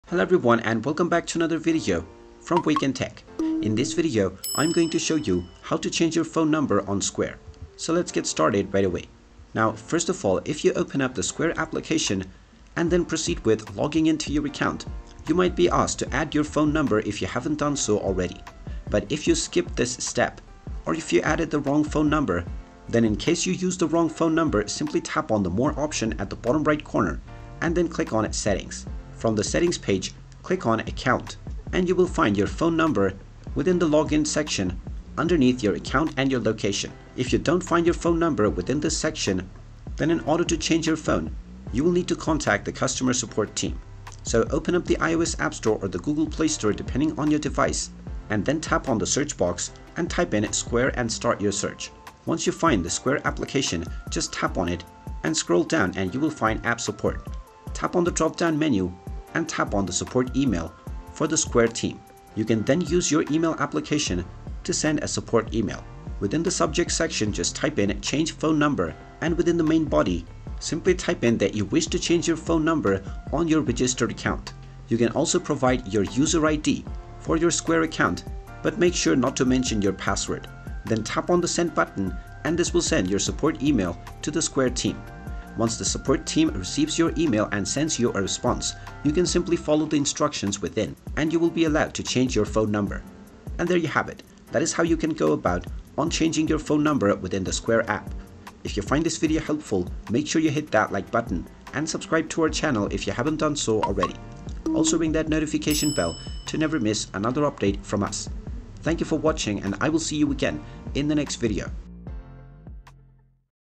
How to Change Your Phone Number on Square? Update Square Phone Number Tutorial (2024)
If you're a Square user looking to change your phone number, you're in the right place! In this video tutorial, we'll guide you through the simple steps to update your phone number on Square.